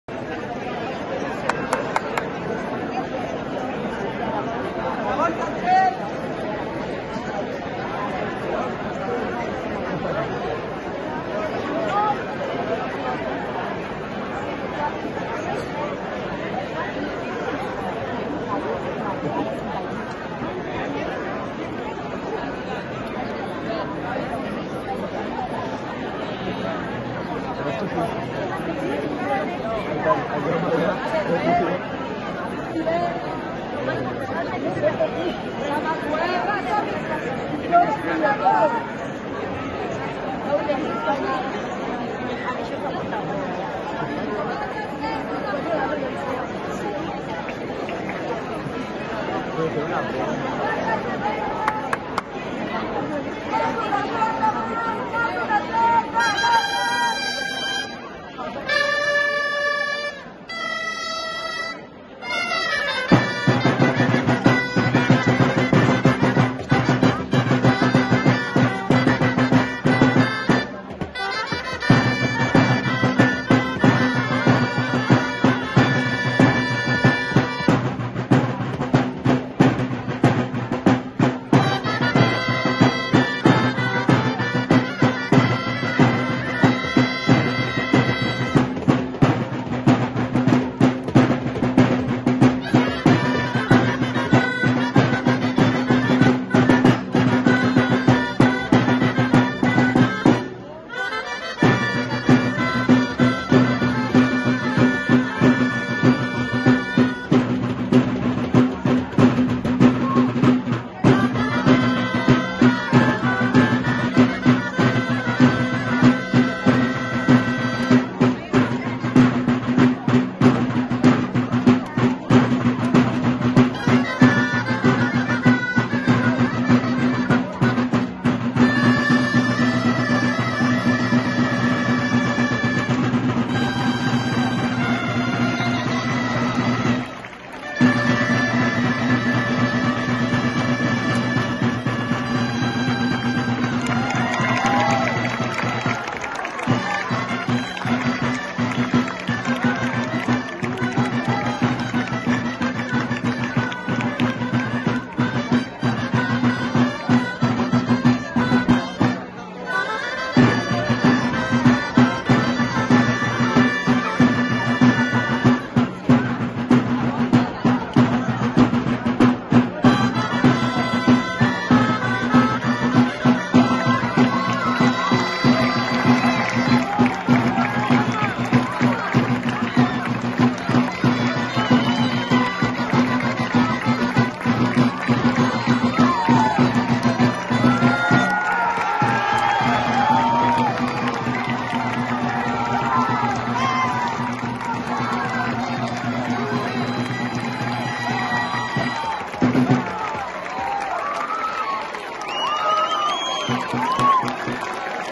7de7 dels Castellers de Caldes per la seva Festa Major
7de7 dels Castellers de Caldes per la diada de Festa Major de Caldes de Montbui, el 12 d'octubre de 2024.

(Imatges cedides pels Castellers de Caldes)